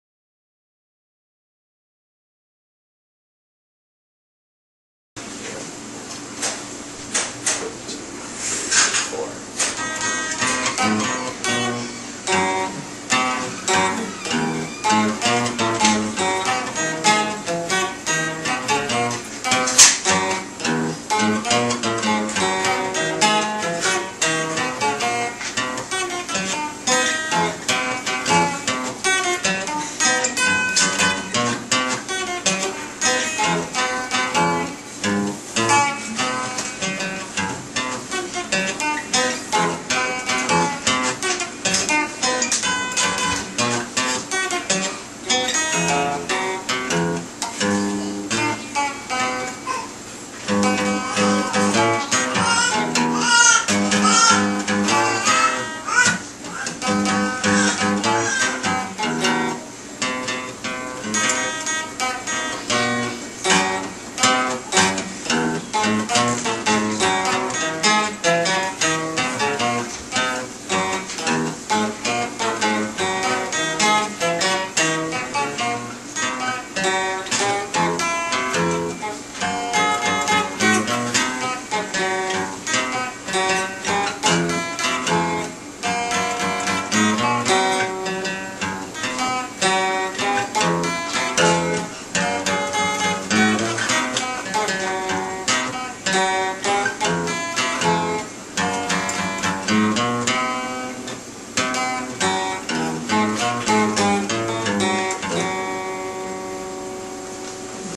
Student Peter Fontana
Davis Guitar Studio: Instructor Jeff Davis
Student Guitar Recital April 23, 2010
The Sweet Afton Tea Room: Plymouth Michigan
Studying Guitar since July 2009